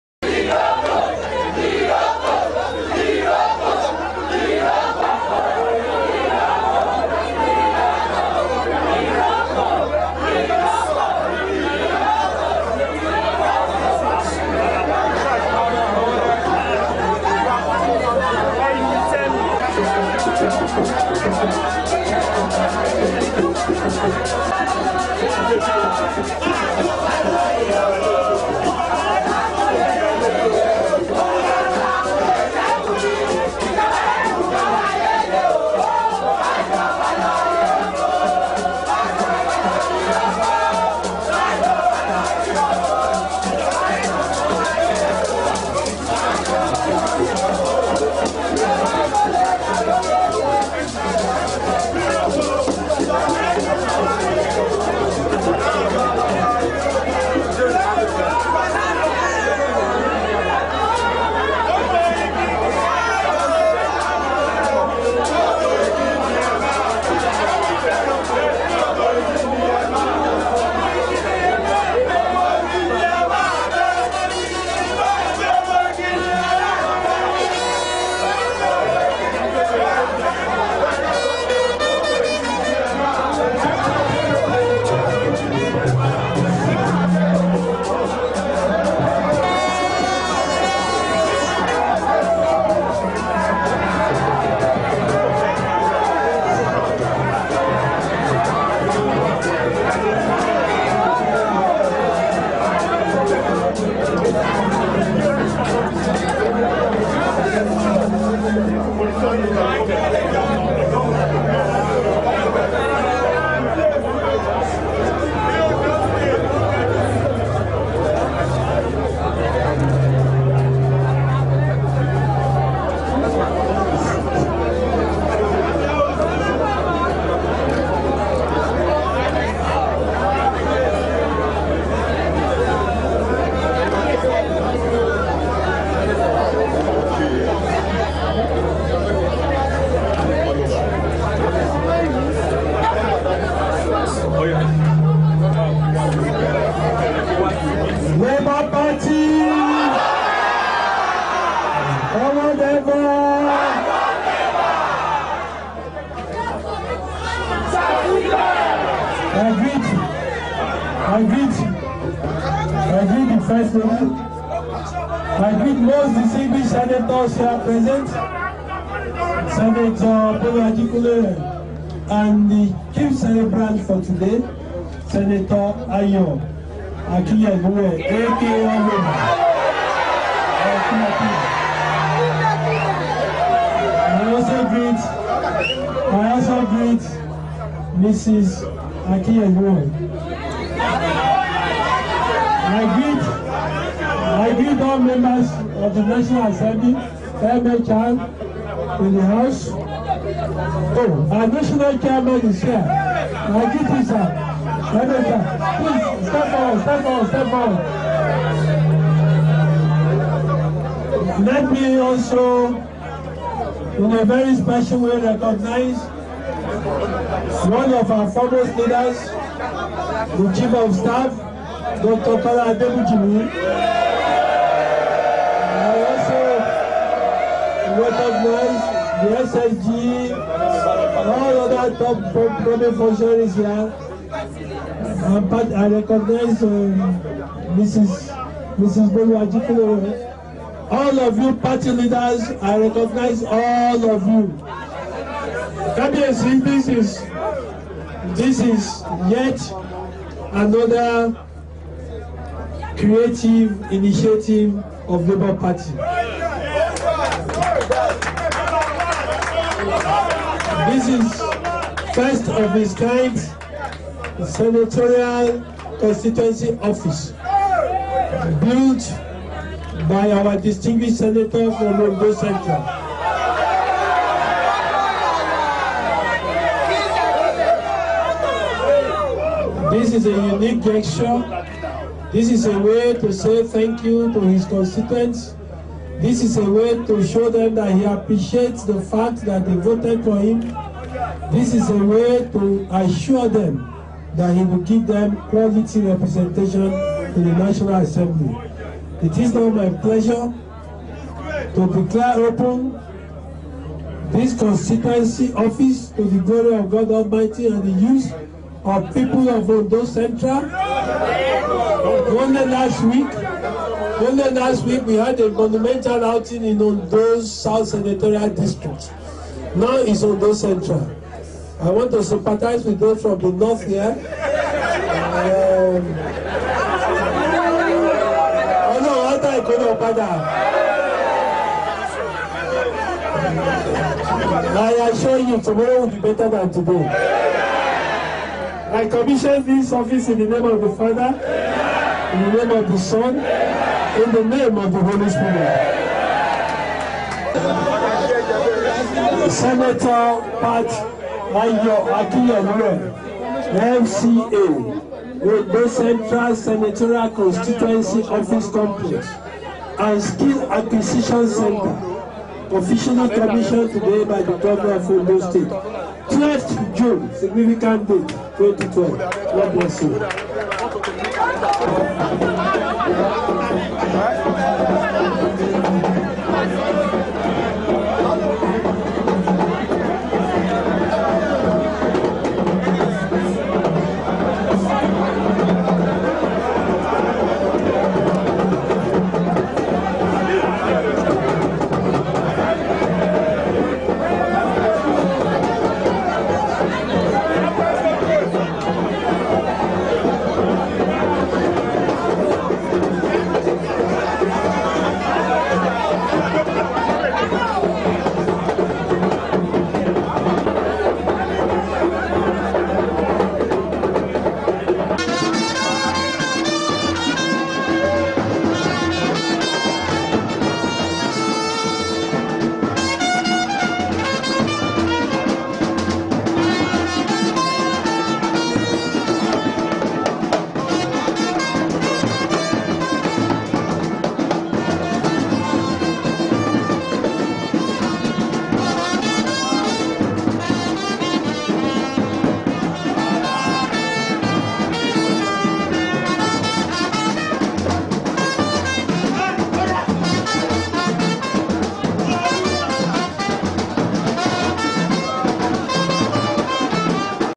Official Commissioning of Ondo Central Senatorial Constituency Office
Official Commissioning of Ondo Central Senatorial Constituency Office of Distinguished Senator Ayo Akinyelure by Governor Olusegun Mimiko of Ondo State on June 12, 2012.